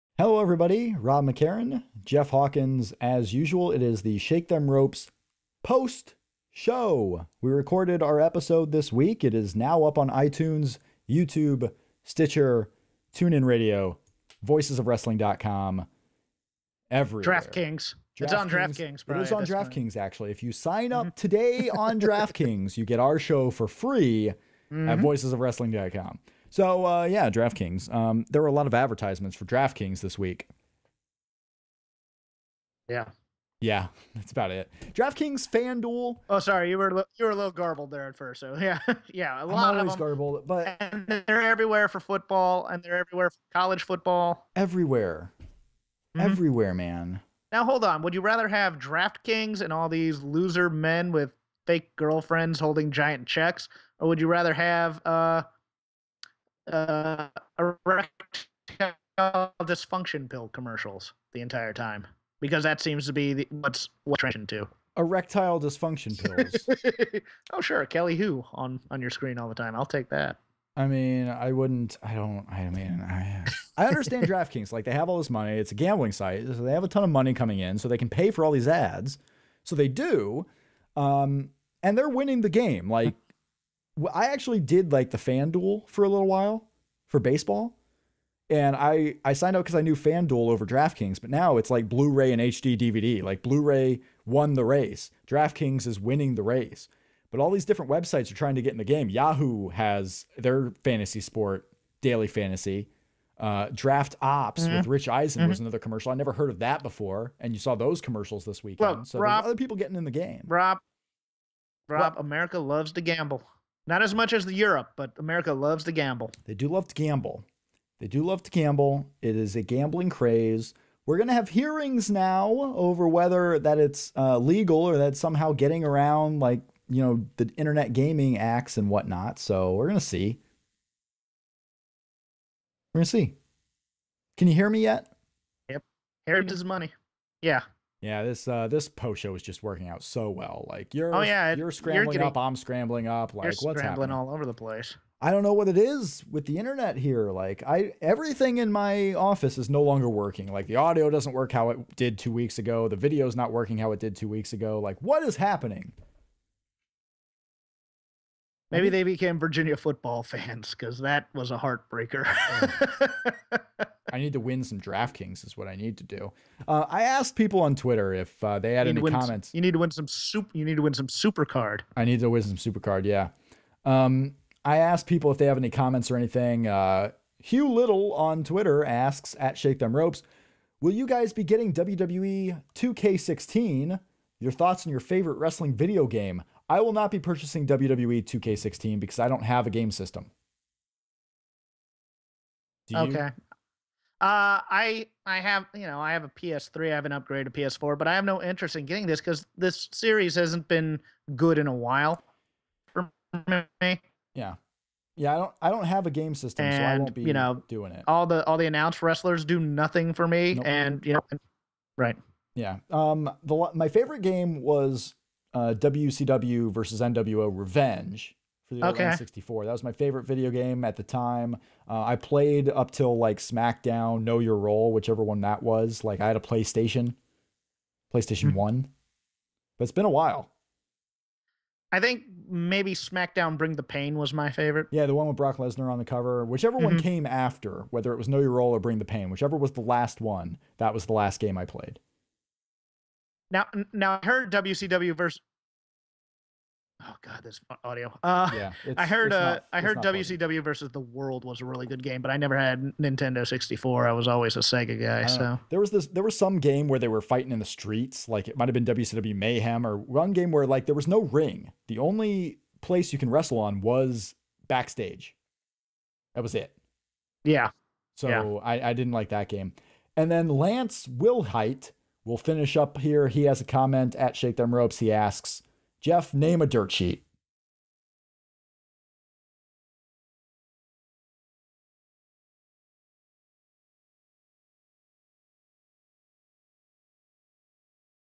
Draft Kings, Video fails | STR Post Show (9/18/15)
STR Video Post Show for September 18, 2015 - We talk the abundance of Draft Kings ads this week, answer Twitter questions from @ShakeThemRopes, and Rob's camera fails.